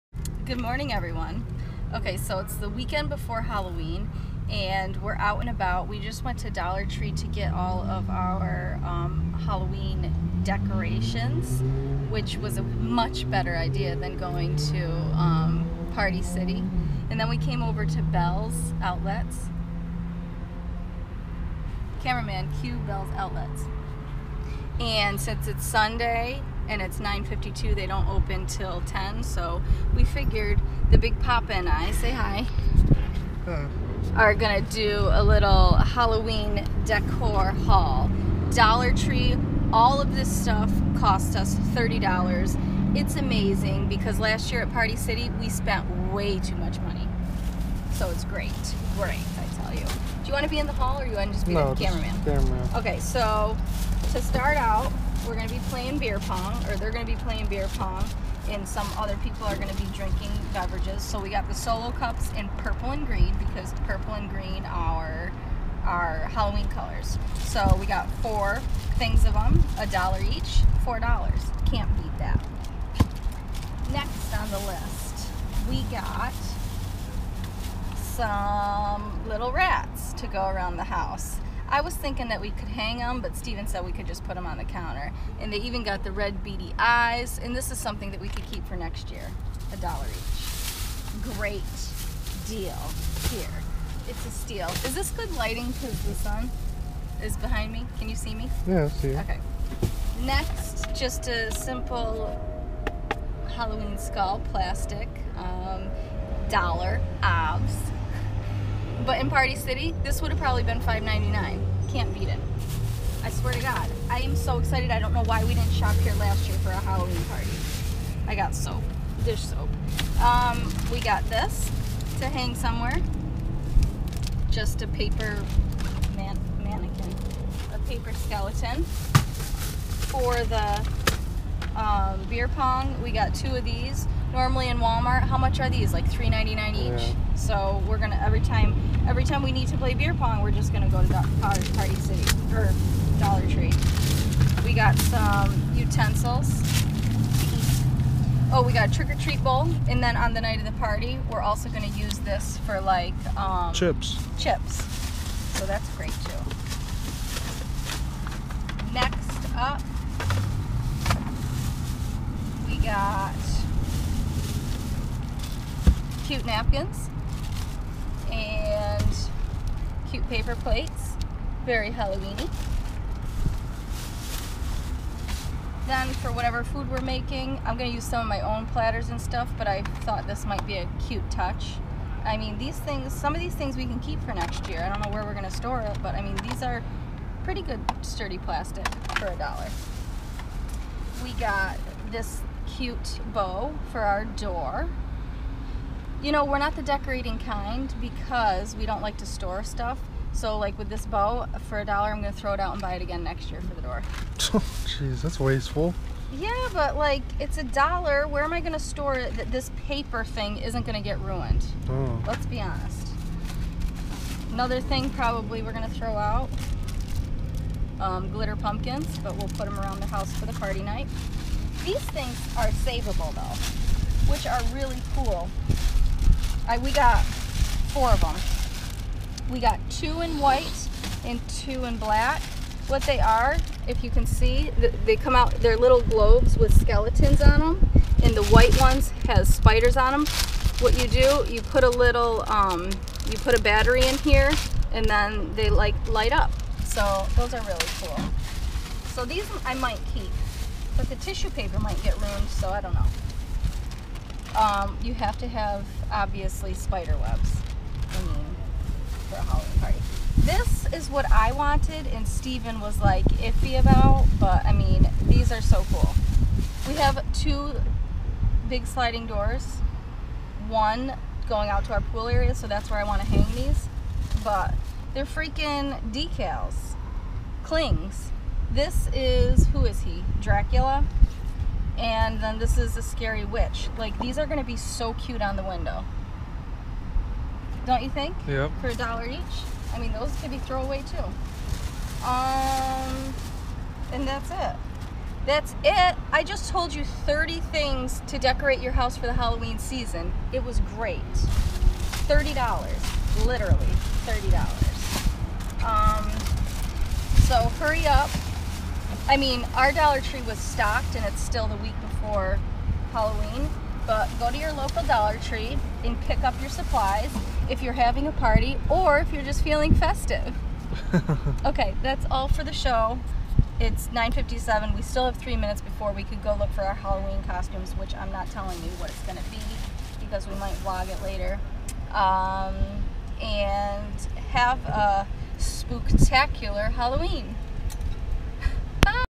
DOLLAR TREE HALLOWEEN DECOR HAUL
It’s Halloween season! I just picked up some decor items from the DOLLAR TREE for our Halloween Party that I’d like to share with you!